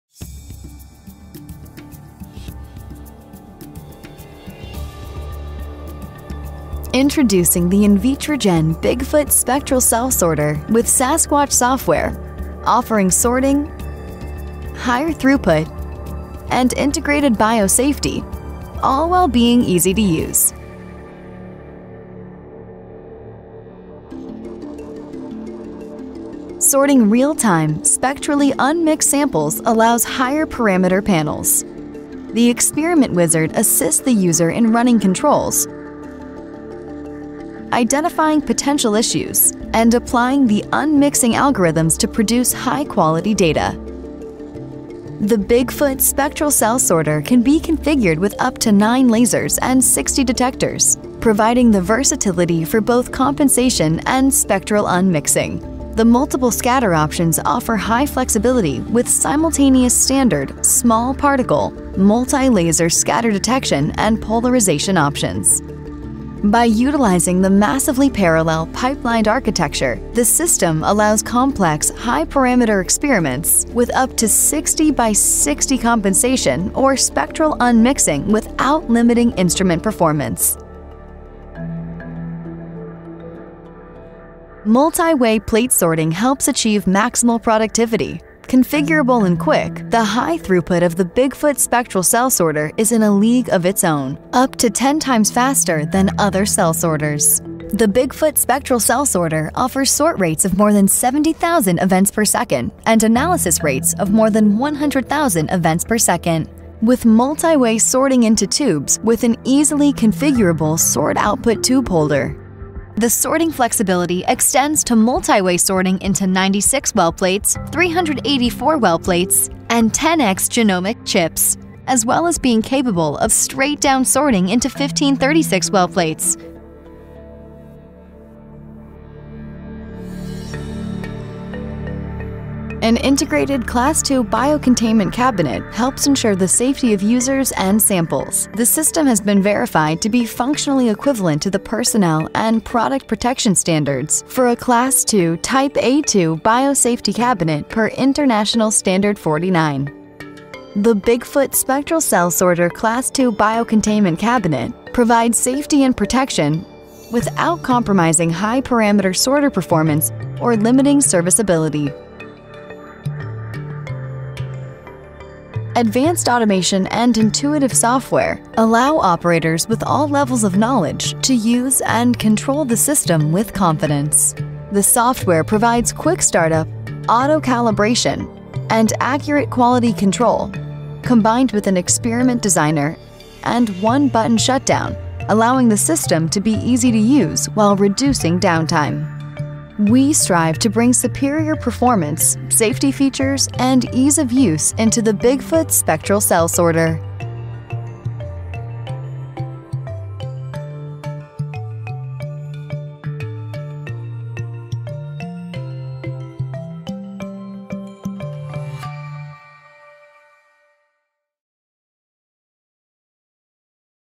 Invitrogen Bigfoot Cell Sorter - Overview
Introducing The Invitrogen Bigfoot Spectral Cell Sorter with Sasquatch Software (SQS) offering spectrally unmixed sorting, higher throughput, and integrated biosafety all while being easy to use.

Sorting real-time, spectrally unmixed samples allows higher parameter panels. The experiment wizard assists the user in running controls, identifying potential issues, and applying the unmixing algorithms to produce high-quality data. The Bigfoot Spectral Cell Sorter can be configured with up to 9 lasers and 60 detectors, providing the versatility for both compensation and spectral unmixing. The multiple scatter options offer high flexibility with simultaneous standard, small particle, multi-laser scatter detection, and polarization options. By utilizing the massively parallel, pipelined architecture, the system allows complex, high parameter experiments with up to 60 x 60 compensation or spectral unmixing without limiting instrument performance.